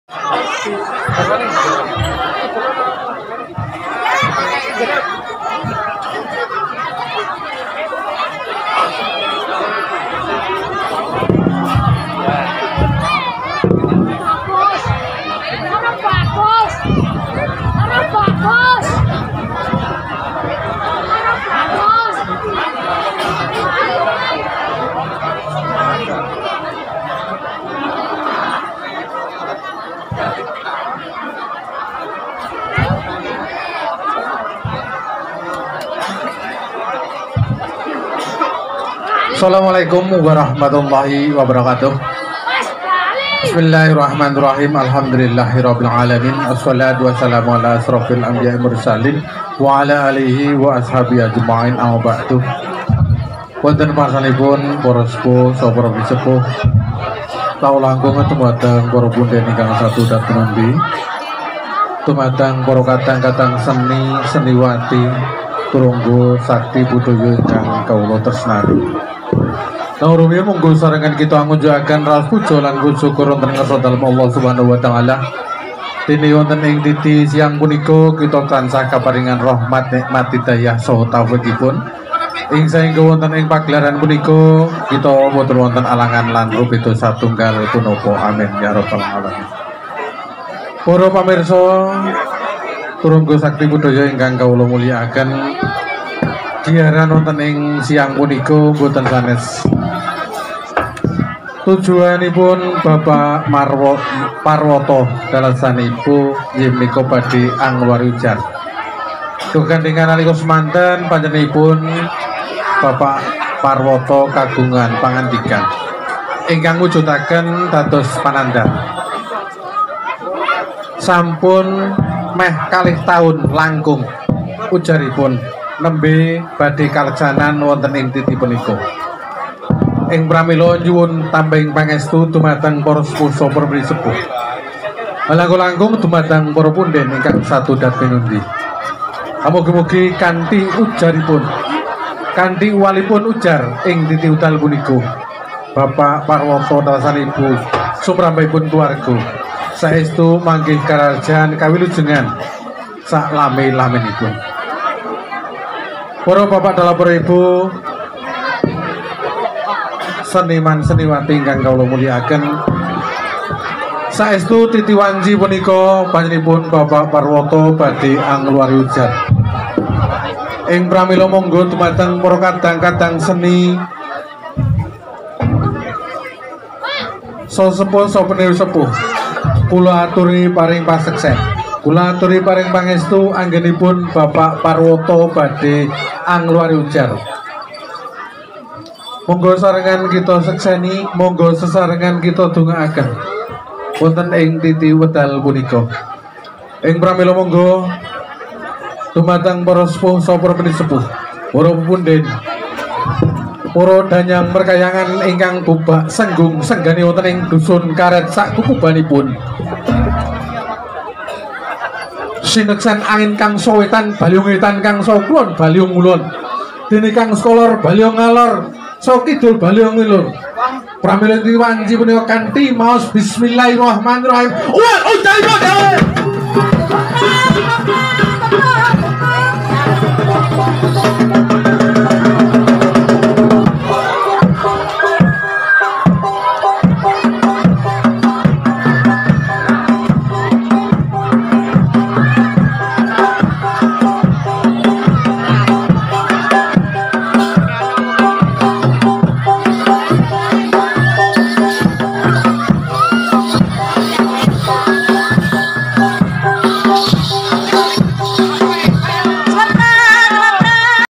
ANGKER !! Tradisi Ngudari Ujar Kaul Bedah Kupat Luar | Melaksanakan Janji Nadzar
wisata budaya magelang Jawa tengah
Indonesian culture life
Indonesian traditional village lifestyle